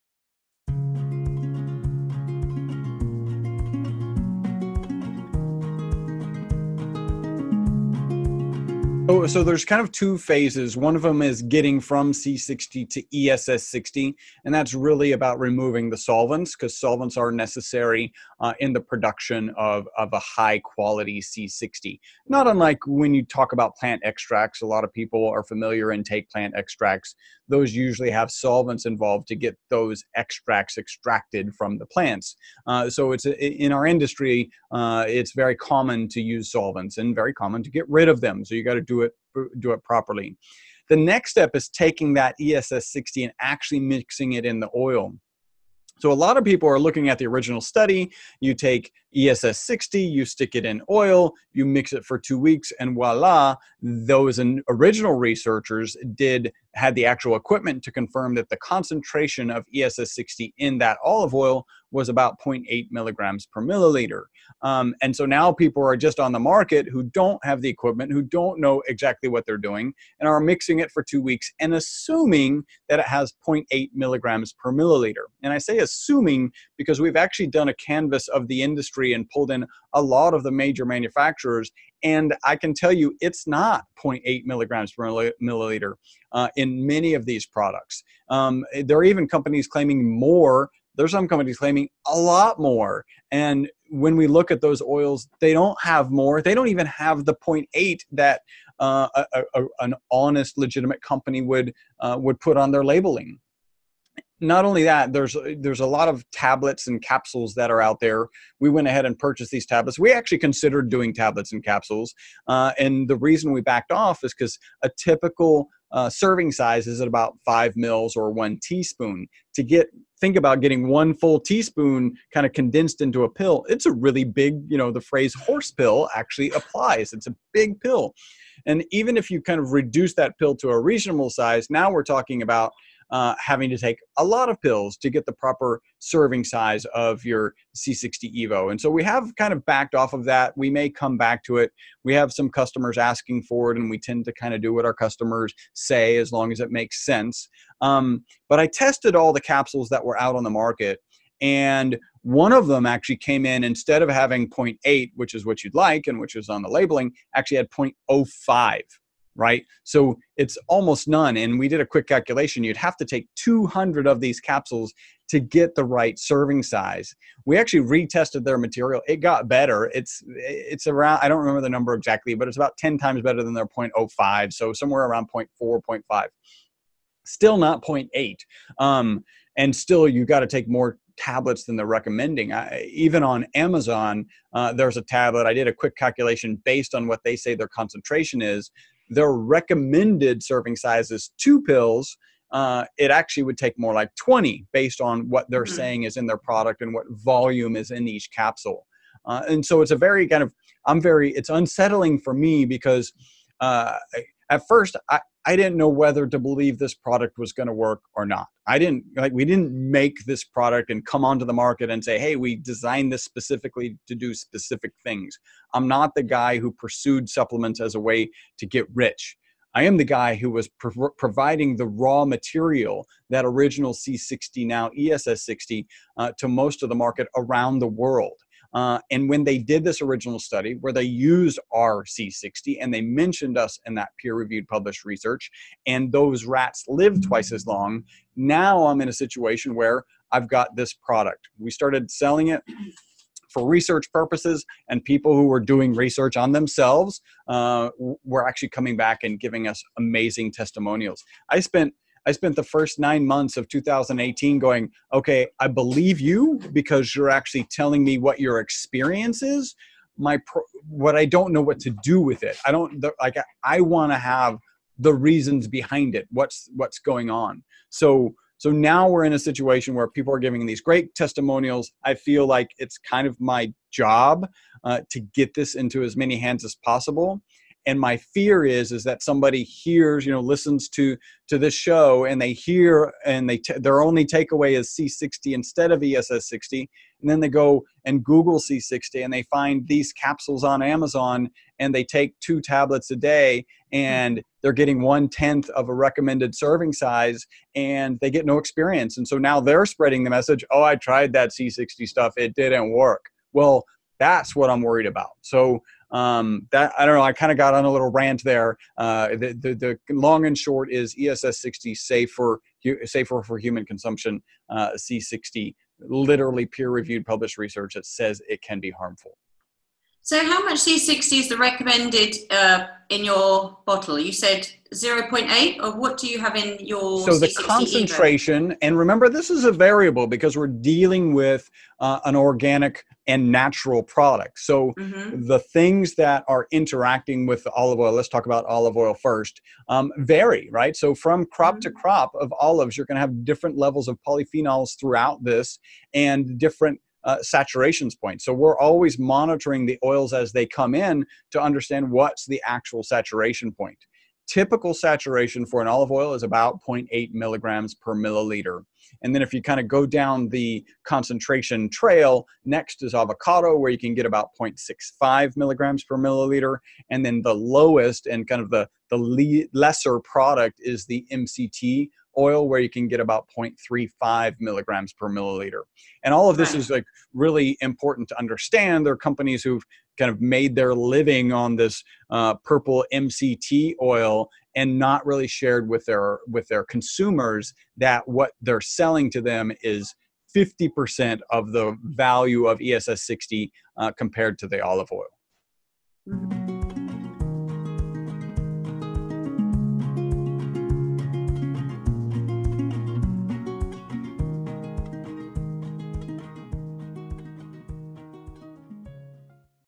Canvassing the Online C60 Market was Surprising - Chris Burres - C60Evo.com and SES Research Labs
Chris Burres said his lab group did a recent product survey, canvassing most of the online C60 products and sellers. Lab results showed very low concentrations of C60 in many well known C60 products. Some labels claimed .8 ml/mg of C60, and had less than .1 mg/ml of C60 in their product. C60 Evo concentrations match what the labels say!

They tested all of the C60 capsule/gelcaps on the market, some of them were the size of horse pills. They found very little C60 in any of them. He said you would need 20-200 capsules to equal 1 teaspoon of C60 Evo. One of the samples he tested was labeled “.8 mg/mls of C60.” When it was tested it had only “.05 mg/mls of C60.” He's concerned about the integrity of the C60 industry after seeing all of these examples.

Chris explains the different saturation points of different oils which are constantly monitored at his lab. Most companies are not explaining to their consumers that they get half as much C60 in coconut oil as  they would get buying C60 mixed into olive oil, no matter how long you stir it.
C60Evo.com